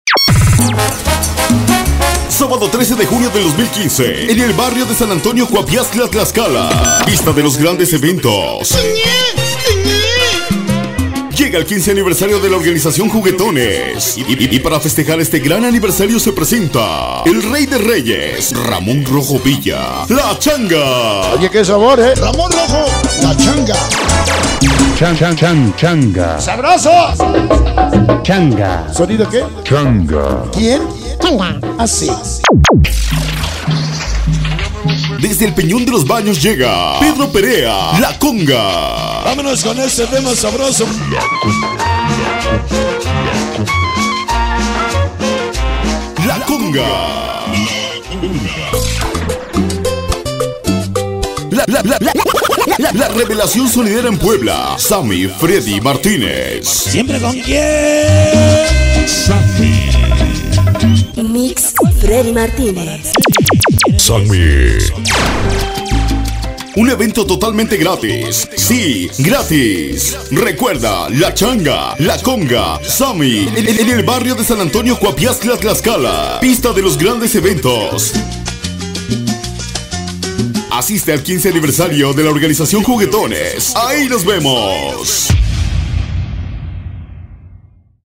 SPOT 15 ANIVERSARIO ORGANIZACION JUGUETONES 2015
SABADO 13 DE JUNIO DEL 2015
EN EL BARRIO SAN ANTONIO CUAPIAXTLA TLAXCALA,PISTA DE LOS GRANDES EVENTOS
LLEGA EL 15 ANIVERSARIO DE ORGANIZACION JUGUETONES
Y PARA FESTEJAR ESTE GRAN ANIVERSARIO SE PRESENTA
EL REY DE REYES,RAMON ROJO VILLA,LA CHANGA
DESDE EL PEÑON DE LOS BAÑOS MEXICO,PEDRO PEREA LA CONGA
LA REVELACION SONIDERA EN PUEBLA,,SAMMY FREDY MARTINEZ
UN EVENTO TOTALMENTE GRATIS,,,SIII GRATIS
RECUERDA LA CHANGA,LA CONGA,SAMMY EN EL BARRIO SAN ANTONIO CUAPIAXTLA TLAXCALA
PISTA DE LOS GRANDES EVENTOS
ASISTE AL 15 ANIVERSARIO,,,ORGANIZACION JUGUETONES,,AHI NOS VEMOSSSSS